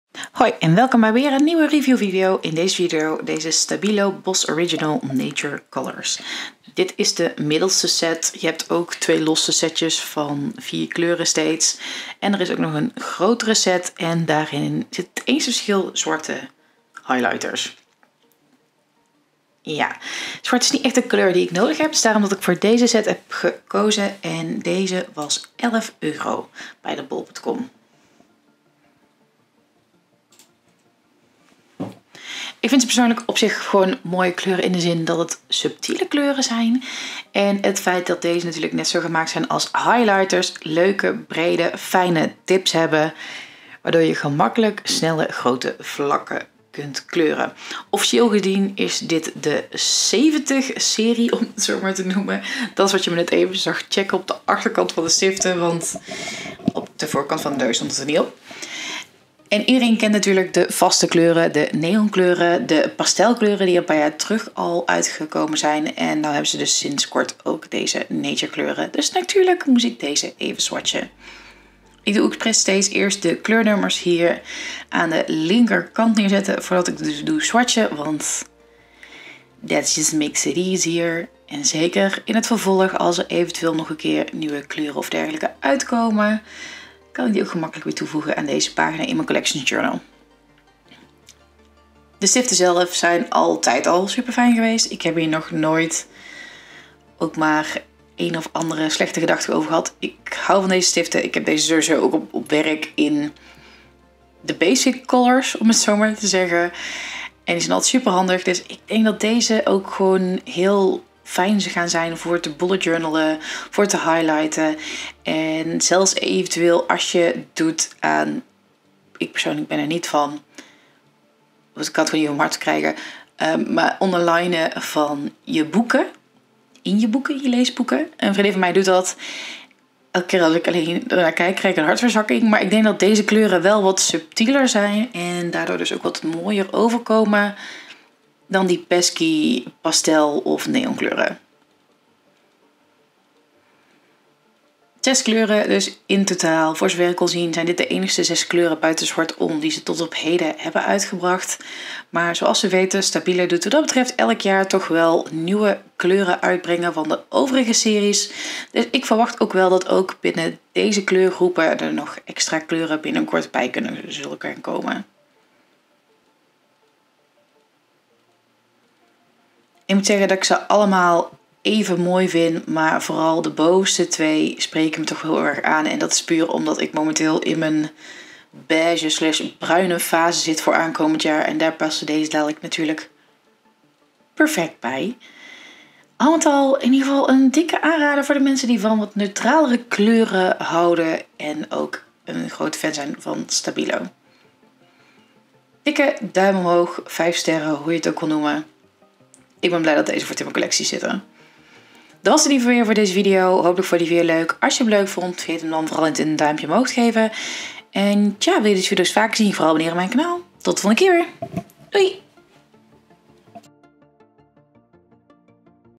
REVIEW || Stabilo nature colors
In deze  video test ik de stabilo highlighters als nature set.  Deze set is momenteel beschikbaar online. Deze set kocht ik voor € 11,00. Tijd om uit te testen.